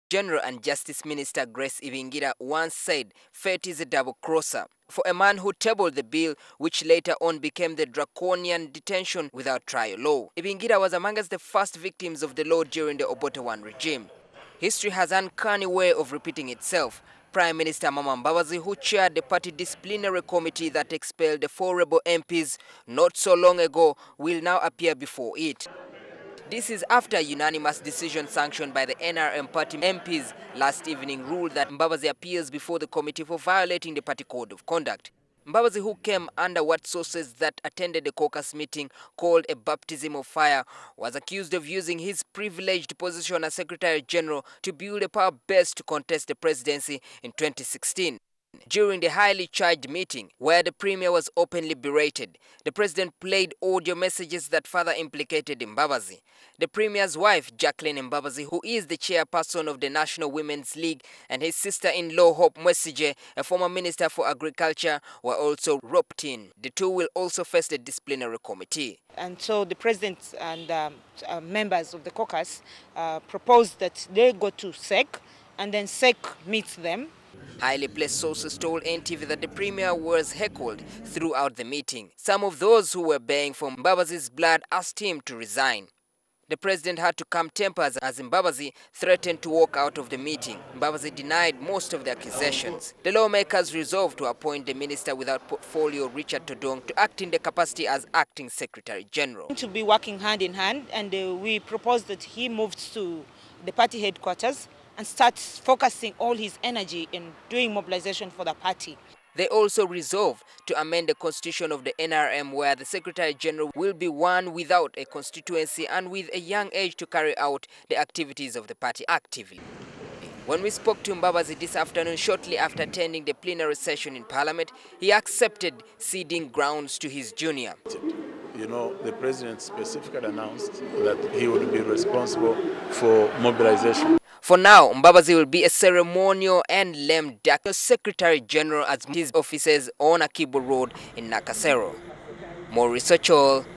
Premier Mbabazi to face NRM disciplinary committee
Prime Minister Amama Mbabazi, also the ruling party Secretary General, is to face the disciplinary committee of the NRM. He is accused of using his privileged position to build a power-base using party structures ahead of the 2016 elections. This decision was reached at after a highly-charged NRM caucus meeting at State House Entebbe, where Mbabazi's perceived covert schemes put him in the crosshairs. His wife Jacqueline Mbabazi who is the Chairperson of the ruling party Women's League and his sister-in-law, Hope Mwesigye, a former minister for Agriculture, will also face the disciplinary committee.